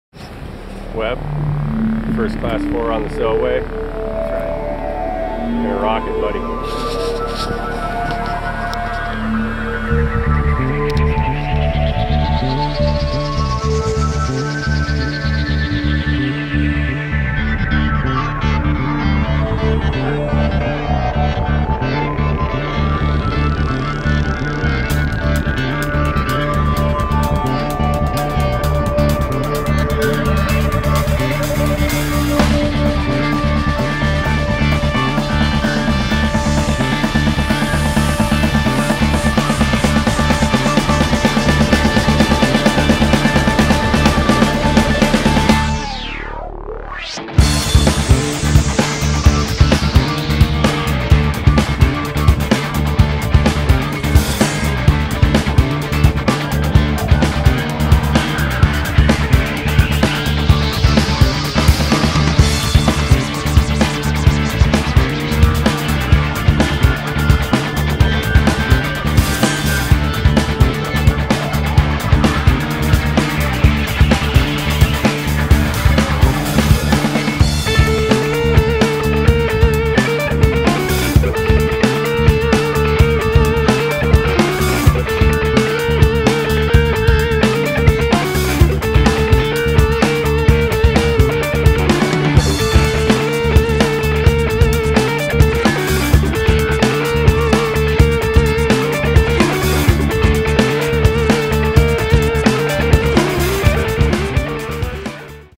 First Timer
My first time on the Selway River rowing my 13' NRS down with good buddy Gordo.. He was alot more confident in me than I was!! Great fun and it wont be the last time on the Selway.